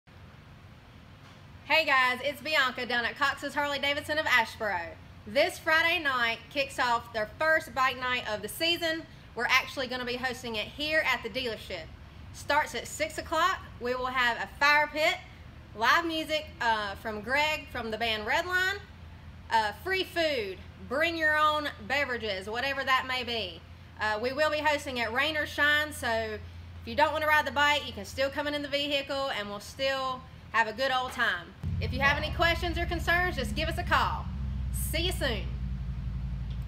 Bike Night at Cox's Harley-Davidson of Asheboro
The weather looks questionable, but we're going to have a party anyway.  Free burgers and dogs while supplies last.  Starts at 6pm.  Music by Greg Russell.

Cox's Harley-Davidson of Asheboro
2795 NC HWY 134